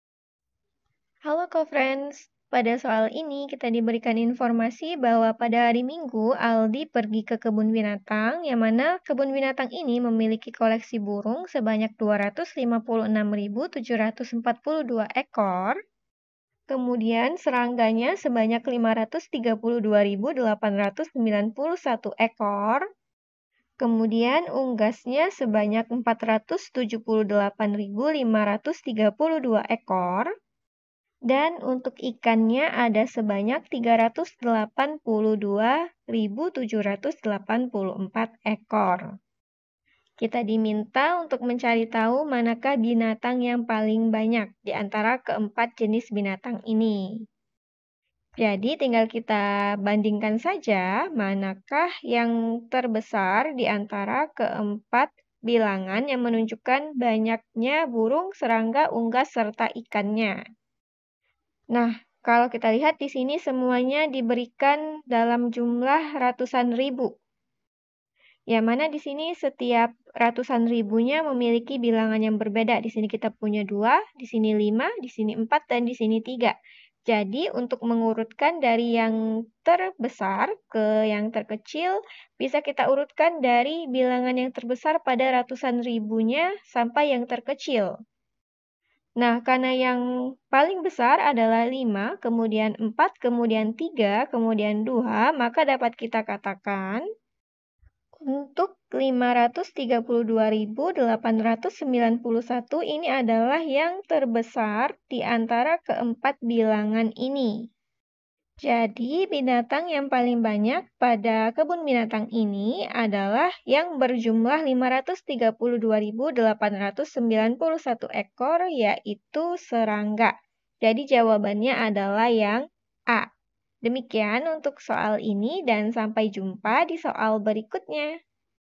Pada hari minggu Aldi pergi ke kebun binatang. Kebun binatang itu memiliki koleksi burung sebanya...
Pada hari minggu Aldi pergi ke kebun binatang. Kebun binatang itu memiliki koleksi burung sebanyak 256.742 ekor. Serangga sebanyak 532.891 ekor. Unggas sebanyak 478.532 ekor, dan ikan sebanyak 382.784 ekor.
Berdasarkan data tersebut, binatang yang paling banyak adalah...

Topik atau Materi: Bilangan Bulat dan Lambangnya - BILANGAN BULAT DAN PECAHAN - BILANGAN - Matematika Kelas 7 SMP - K-13 revisi

00:00 : Bedah Soal
01:00 : Konsep, rumus dan pengertian Bilangan Bulat dan Lambangnya
01:18 : Langkah penyelesaian soal
01:08 : Jawaban akhir
01:38 : Penutup